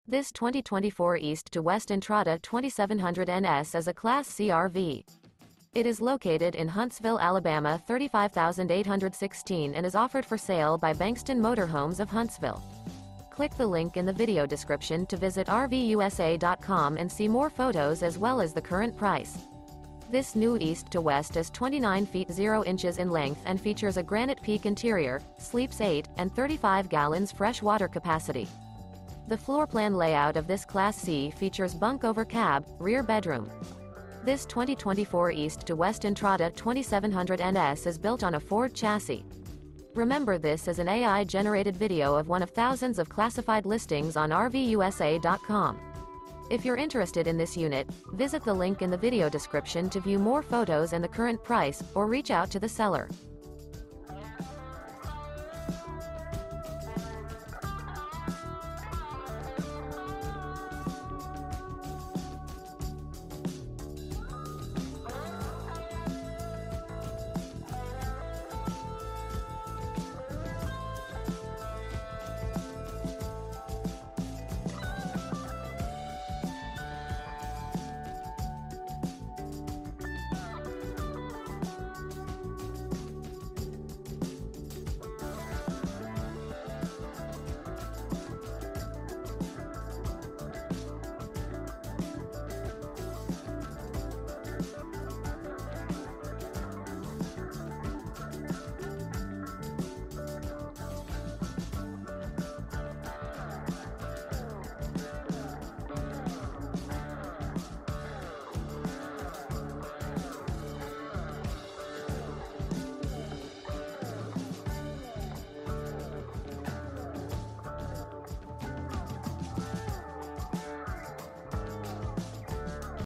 Wonderful 2024 East to West Entrada Class C RV For Sale in Huntsville, AL | RVUSA.com
This 2024 East to West Entrada 2700NS is a Class C RV. It is located in Huntsville, AL 35816 and is offered for sale by Bankston Motor Homes of Huntsville. This New East to West Is 29' 0" in length and features a GRANITE PEAK interior, sleeps 8, and 35 gal fresh water capacity. The floorplan layout of this Class C features Bunk Over Cab, Rear Bedroom. This 2024 East to West Entrada 2700NS is built on a Ford chassis.